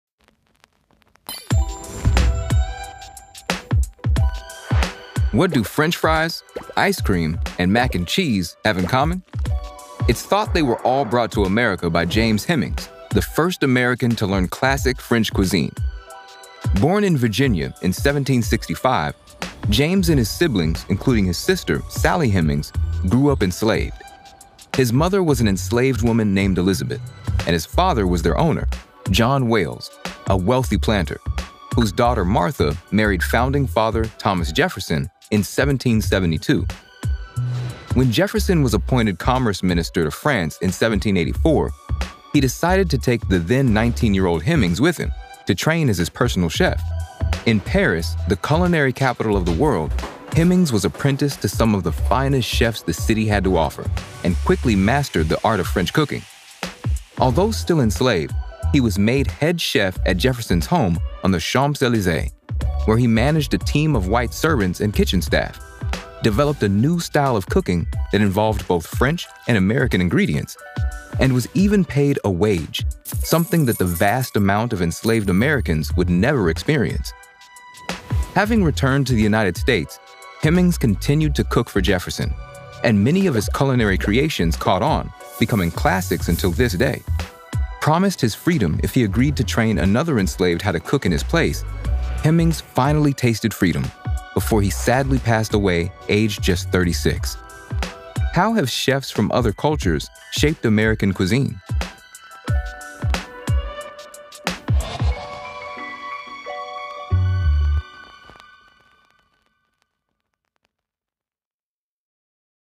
James Hemings: The Enslaved Chef Who Revolutionized American Cuisine
James Hemings was an enslaved man and the first American to learn classic French cuisine. He helped popularise many of the dishes you know and love today.